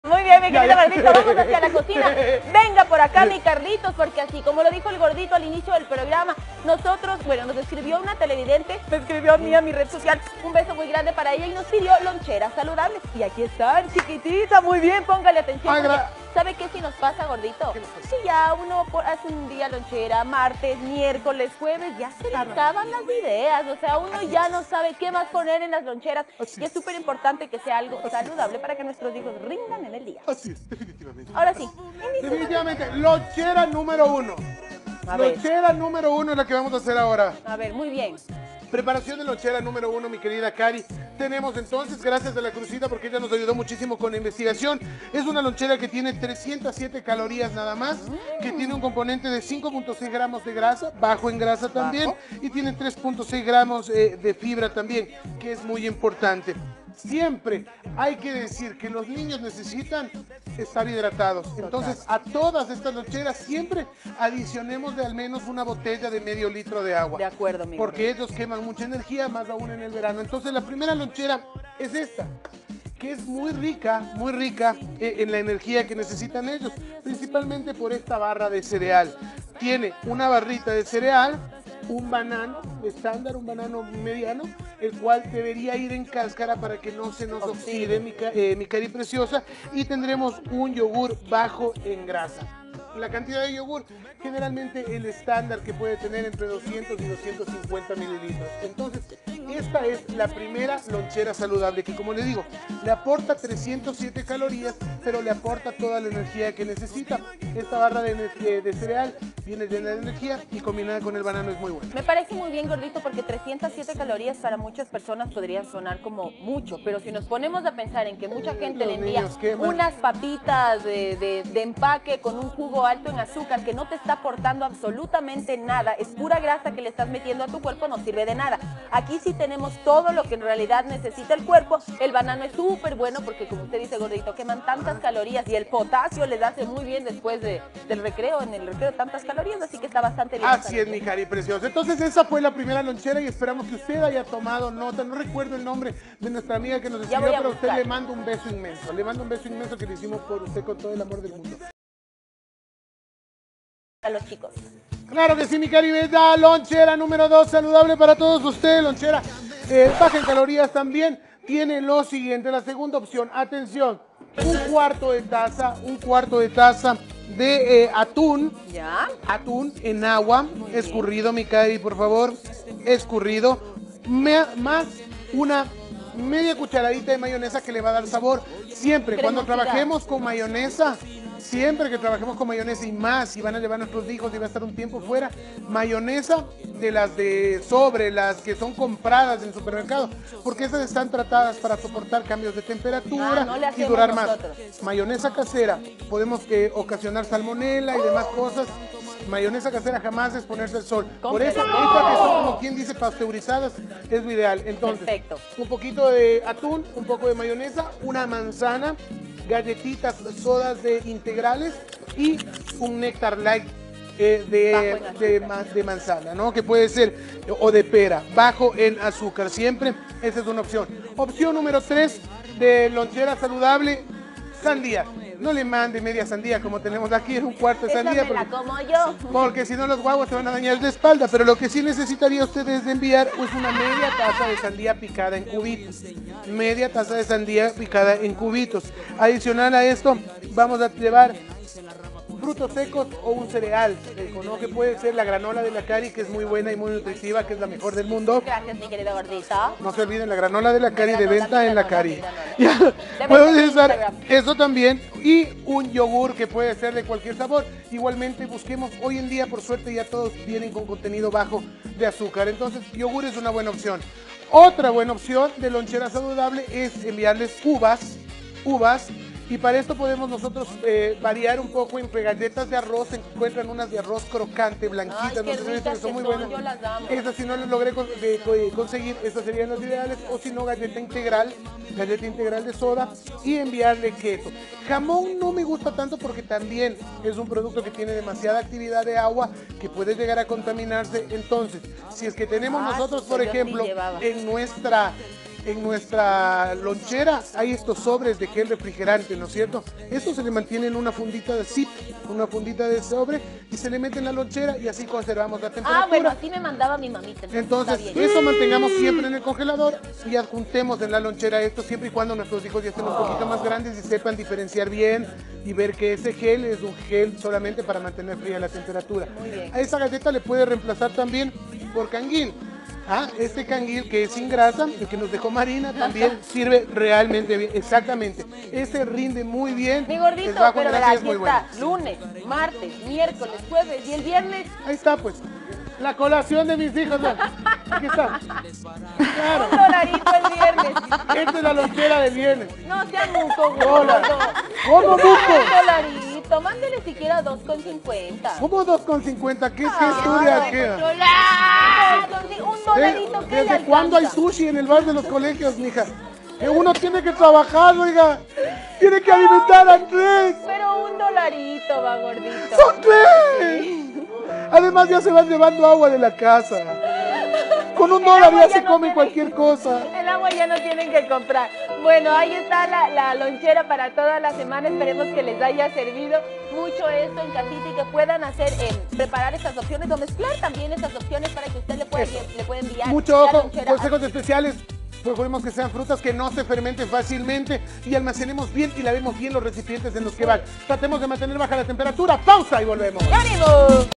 Revisa algunas opciones de ''loncheras nutritivas''
Hoy te presentamos algunas opciones de "loncheras nutritivas" ideales para que nuestros niños lleven a sus colegios y se puedan alimentar de forma saludable.